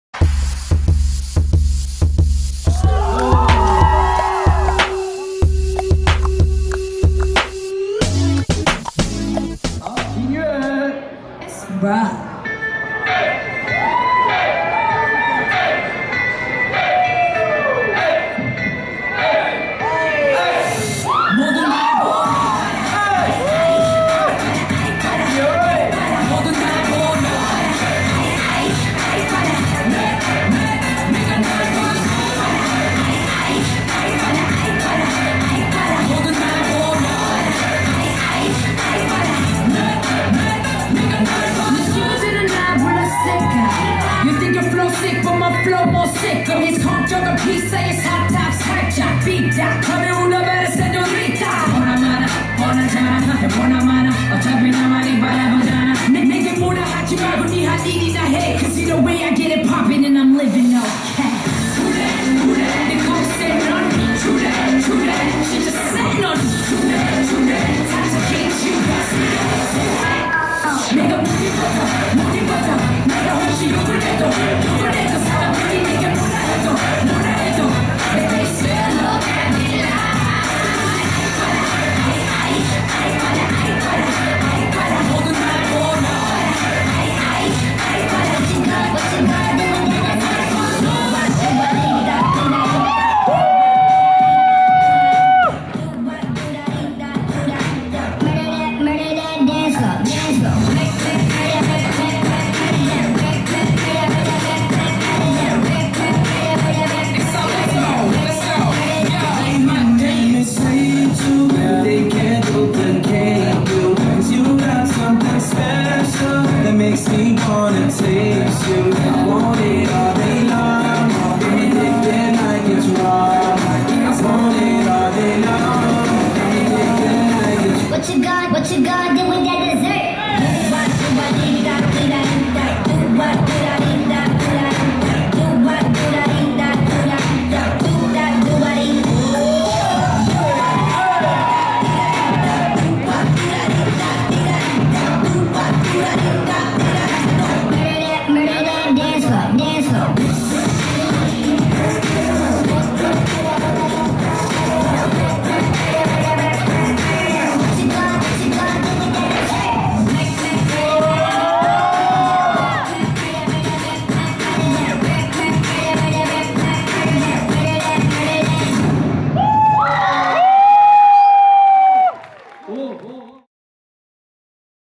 曼斯特成果發表VOL.2 / 7.INFECTION培訓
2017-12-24 曼斯特成果發表VOL.2
舞碼：FREE STYLE
團隊：INFECTION培訓
演出人員：子瑜、雷叮、奕萱、彥妏